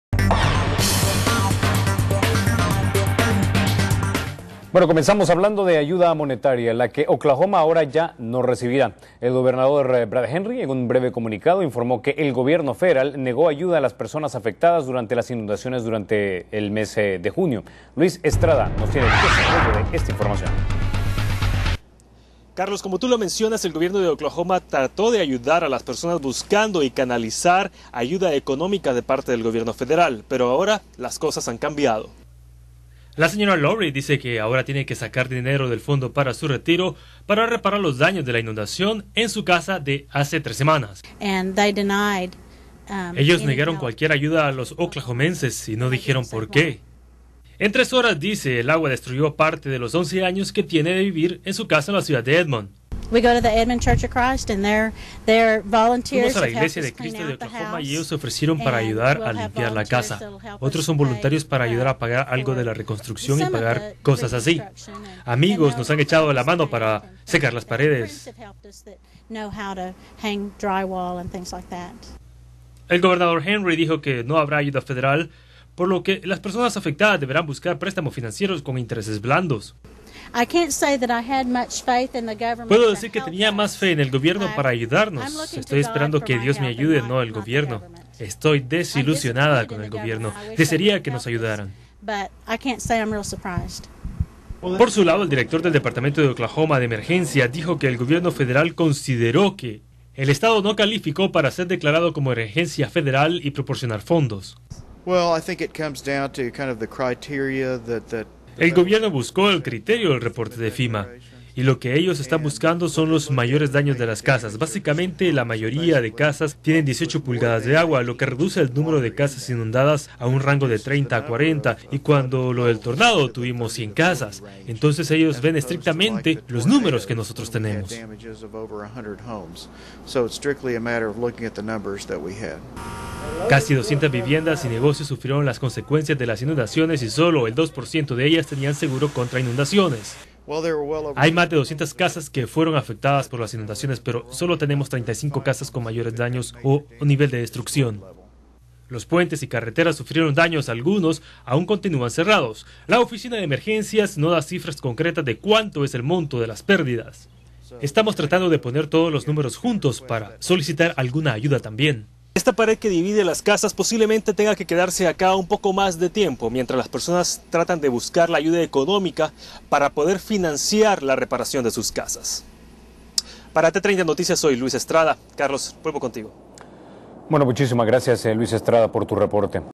07 06 2010. NO HABRA AYUDA FEDERAL PARA AFECTADOS POR INUNDACIONES.
EN OKLAHOMA EL 14 DE JUNIO.